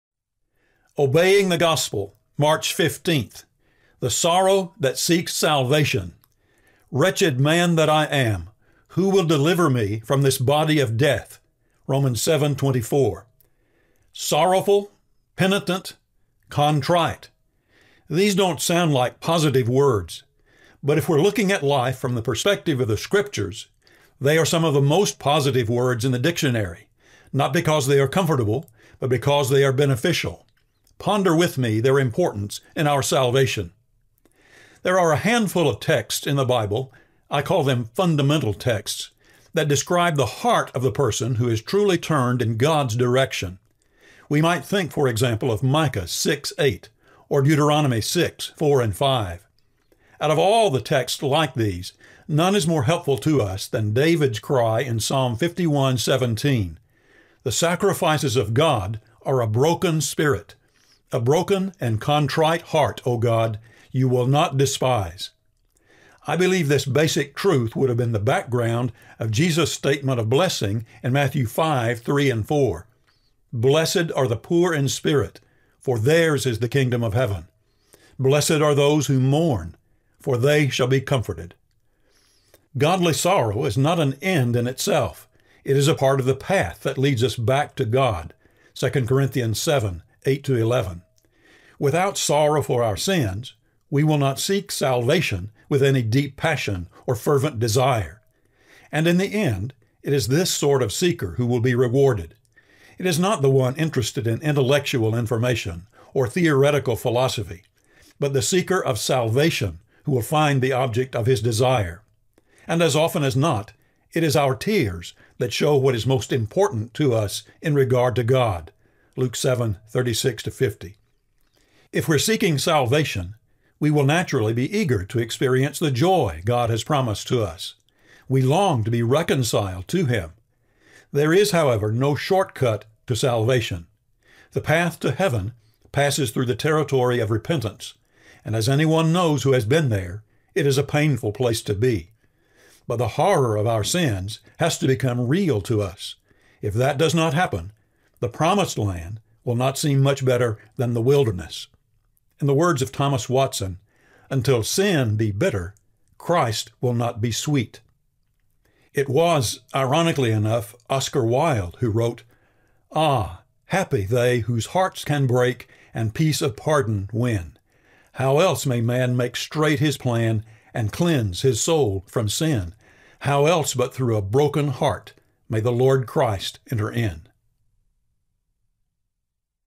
The Sorrow That Seeks Salvation (March 15)
"Wretched man that I am! Who will deliver me from this body of death?" (Romans 7:24).

SORROWFUL. PENITENT. CONTRITE. These don’t sound like positive words. But if we’re looking at life from the perspective of the Scriptures, they are some of the most positive words in the dictionary — not because they are comfortable but because they are conducive to our salvation. Ponder with me their importance.

There are a handful of texts in the Bible (I call them “fundamental” texts) that describe the heart of the person who is truly turned in God’s direction. We might think, for example, of Micah 6:8 or Deuteronomy 6:4,5. Out of all the texts like these, none is more helpful to us than David’s cry in Psalm 51:17: “The sacrifices of God are a broken spirit; a broken and contrite heart, O God, you will not despise.” I believe this basic truth would have been the background of Jesus’ statement of blessing in Matthew 5:3,4: “Blessed are the poor in spirit, for theirs is the kingdom of heaven. Blessed are those who mourn, for they shall be comforted.”

Godly sorrow is not an end in itself; it is a part of the path that leads us back to God (2 Corinthians 7:8-11). Without sorrow for our sins, we will not seek salvation with any deep passion or fervent desire. And in the end, it is this sort of seeker who will be rewarded. It is not the one interested in intellectual information or theoretical philosophy, but the seeker of salvation who will find the object of his desire. And as often as not, it is our tears that show what is most important to us in regard to God (Luke 7:36-50).

If we’re seeking salvation, we will naturally be eager to experience the joy God has promised to us. We long to be reconciled to Him. There is, however, no shortcut to salvation. The path to heaven passes through the territory of repentance, and as anyone knows who has been there, it is a painful place to be. But the horror of our sins has to become real to us. If that does not happen, the Promised Land will not seem much better than the wilderness. “Until sin be bitter — Christ will not be sweet” (Thomas Watson).

Ah! happy they whose hearts can break
And peace of pardon win!
How else may man make straight his plan
And cleanse his soul from sin?
How else but through a broken heart
May the Lord Christ enter in?
(Oscar Wilde)